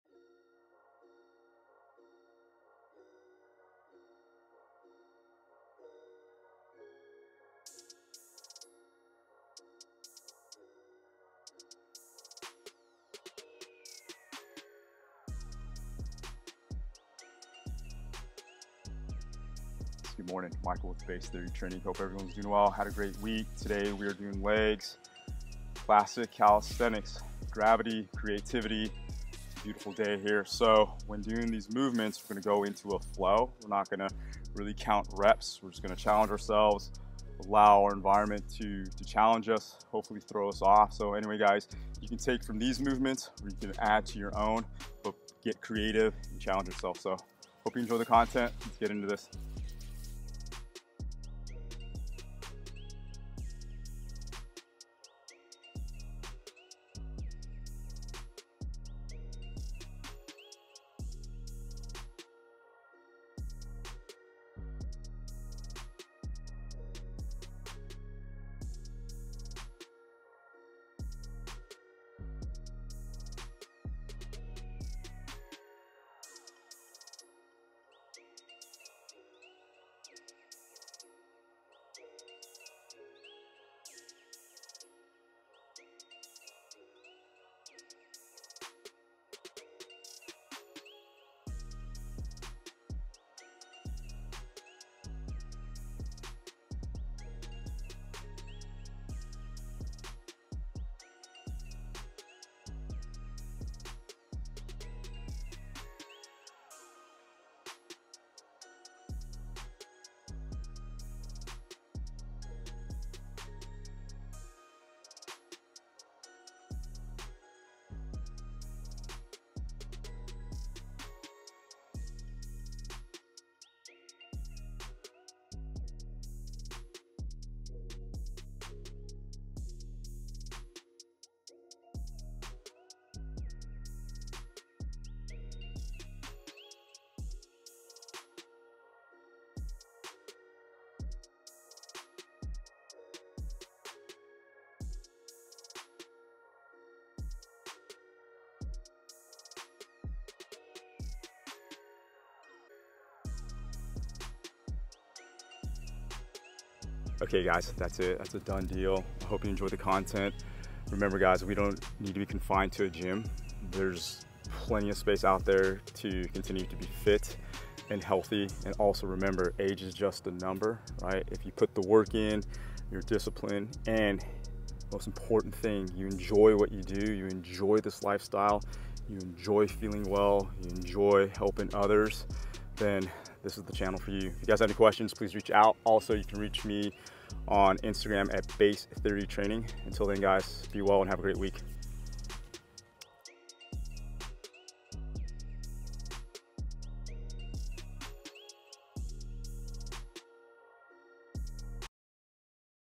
Build Leg Muscle with Bodyweight Anytime Anywhere
This week’s episode is a fun and functionally effective training flow. Linking together multiple movements for an athletic training stimulus.

Please reach out with any questions.

Be well,
Michael

Training Instructions:

Jump Rope: 5 x 2 Minute Intervals
Reverse Hypers: 15 Reps x 3 Sets
Hollow Body Pulses: 20 Reps x 3 Sets

Movement Flow:
Sissy Squat: 4
RDL: 1
Pistol Squat: 1
Shrimp Squat: 1
4 Sets Per Leg
Duck Walk: 10 Paces

*Rest 2 minutes+ rest between